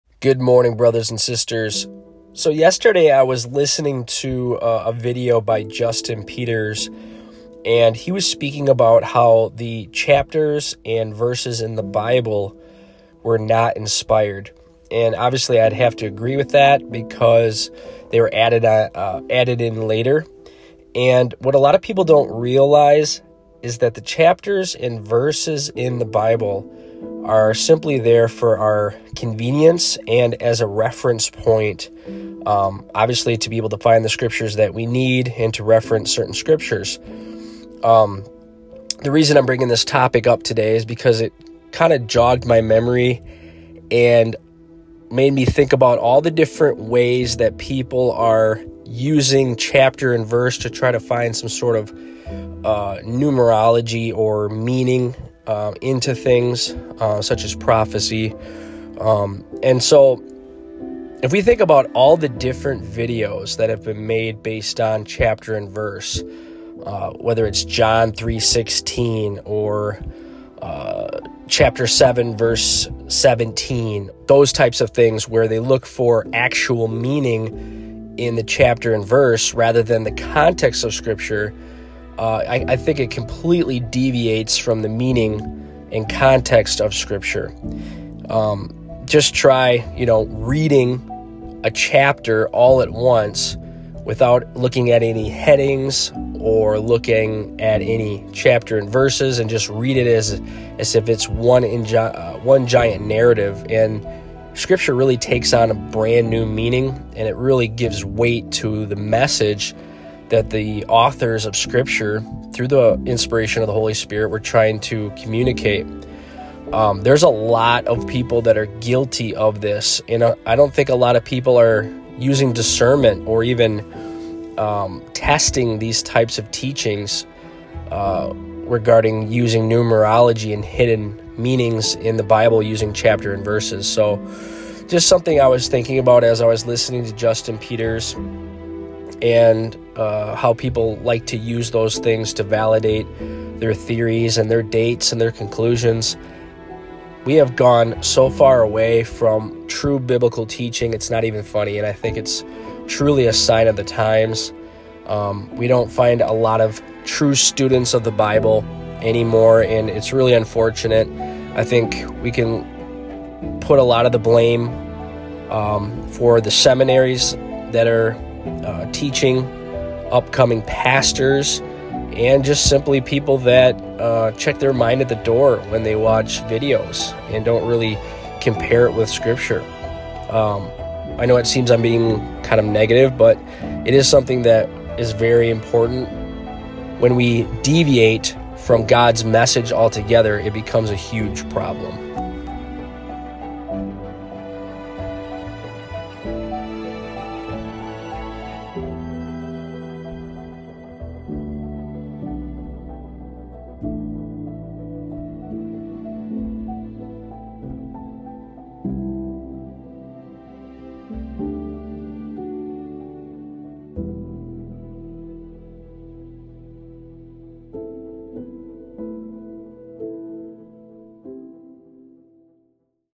The Bible Chapters & Verses aren’t Inspired? 📖
We need to be careful reading meaning into the chapters & verses in the Bible!!

The chapter divisions commonly used today were developed by Stephen Langton, an Archbishop of Canterbury. Langton put the modern chapter divisions into place in around A.D. 1227. The Wycliffe English Bible of 1382 was the first Bible to use this chapter pattern. Since the Wycliffe Bible, nearly all Bible translations have followed Langton’s chapter divisions.

The Hebrew Old Testament was divided into verses by a Jewish rabbi by the name of Nathan in A.D. 1448. Robert Estienne, who was also known as Stephanus, was the first to divide the New Testament into standard numbered verses, in 1555.

We need to speak the truth in love with people are teaching this way! 💯